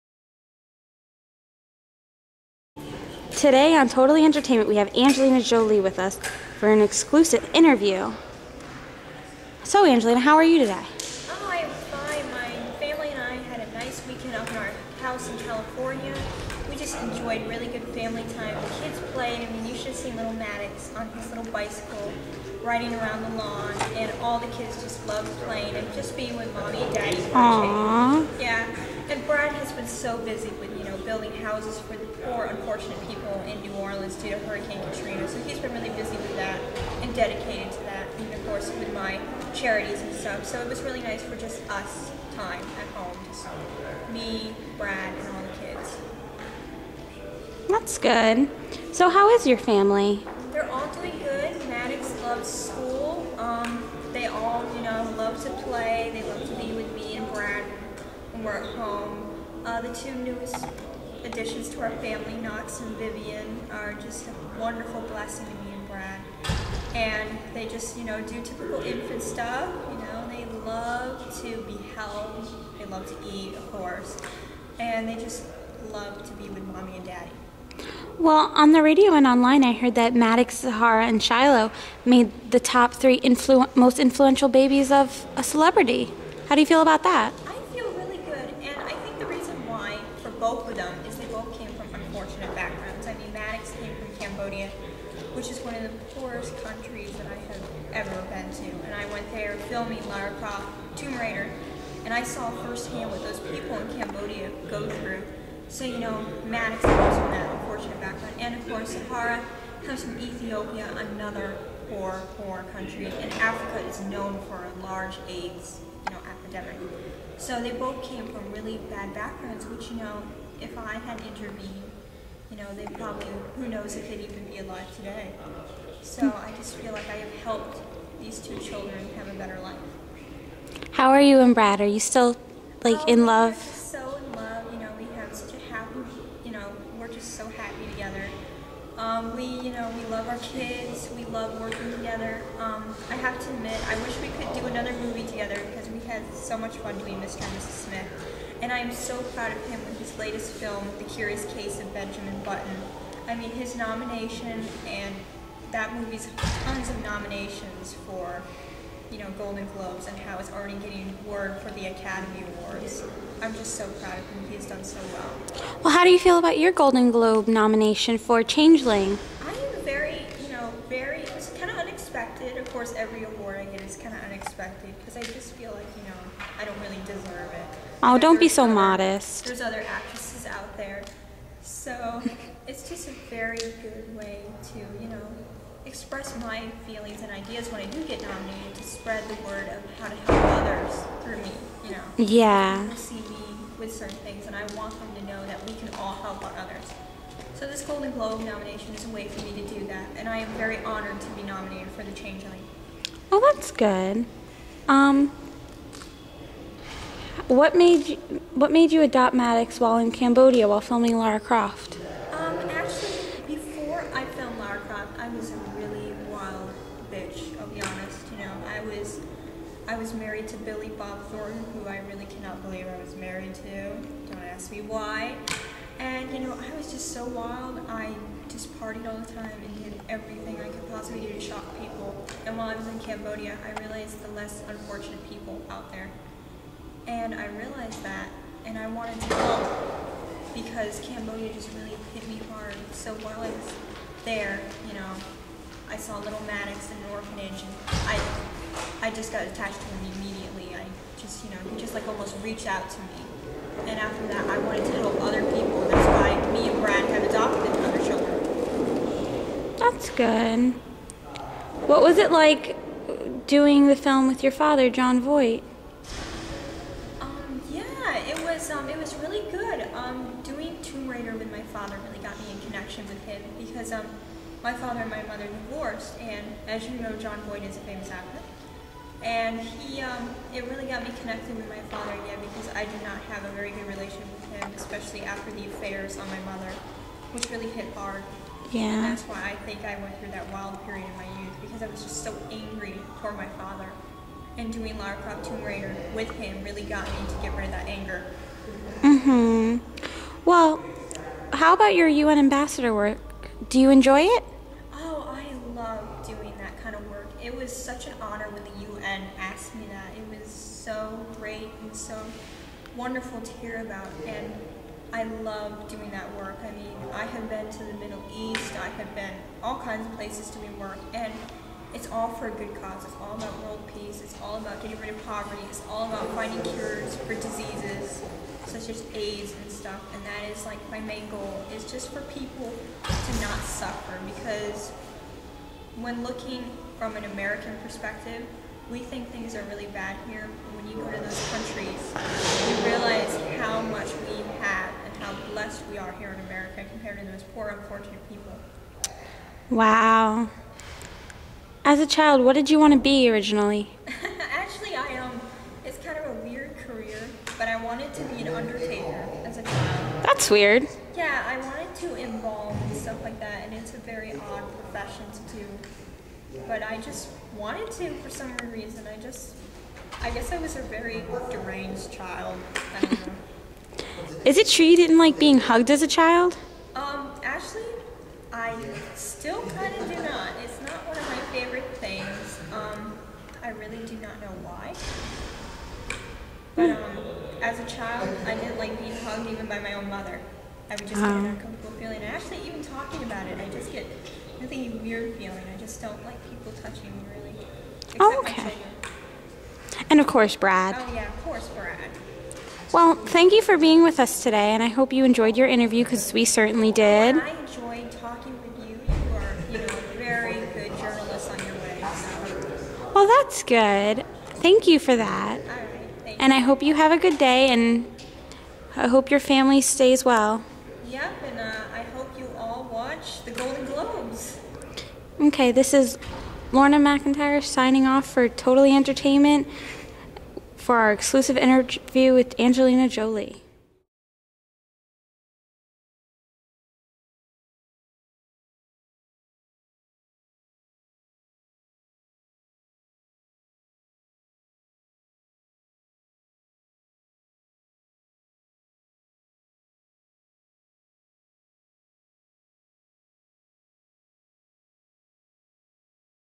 Composition 2 Drama Project
Me playing Angelina Jolie in an interview